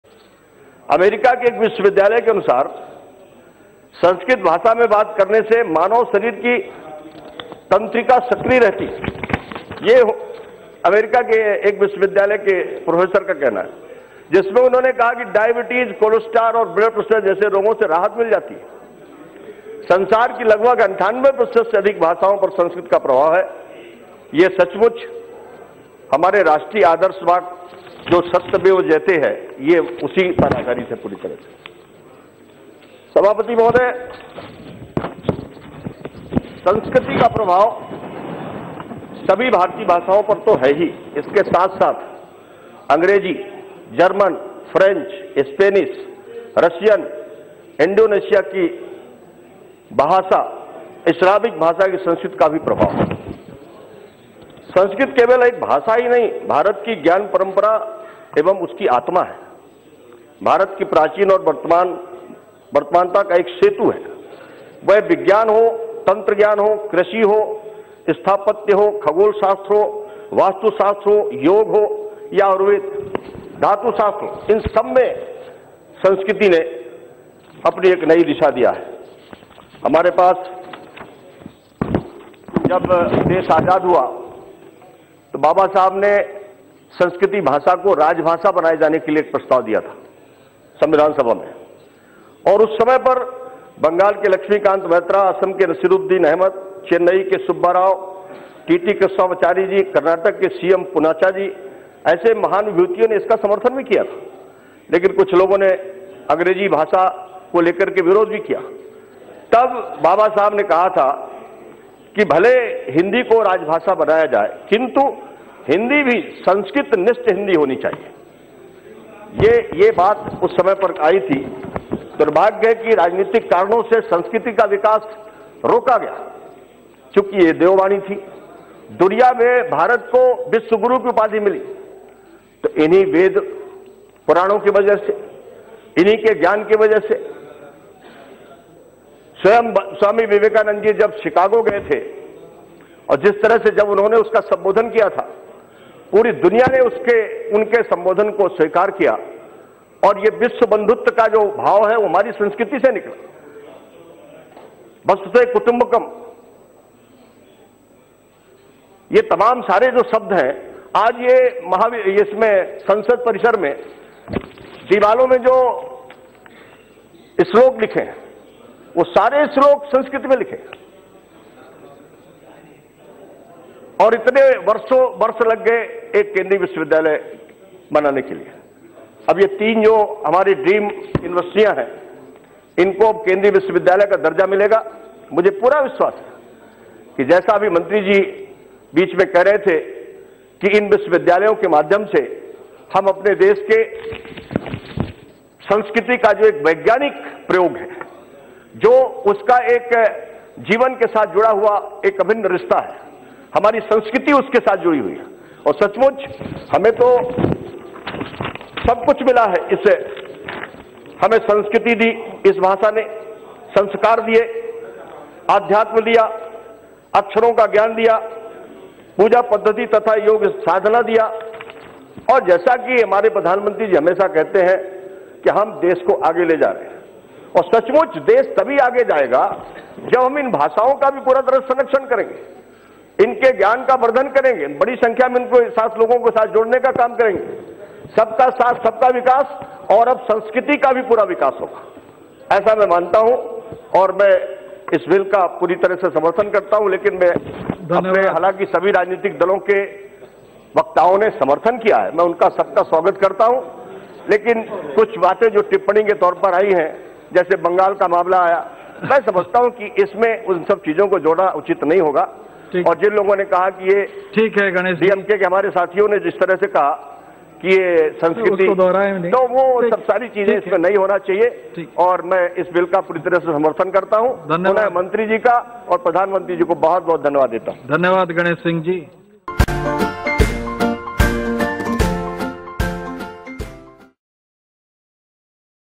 VIRAL VIDEO: BJP MP Ganesh Singh Speech on Sanskrit Language in Loksabha
सारे ज्ञानी एक ही पार्टी मे कैसे हैं ? ये बीजेपी सांसद बोल रहे हैं के संस्कृत बोलने से diabetes और cholesterol नियन्त्रण मे रहता है । भक्तों , बजाओ ताली।

Speaking Sanskrit keeps diabetes, cholesterol at bay: BJP MP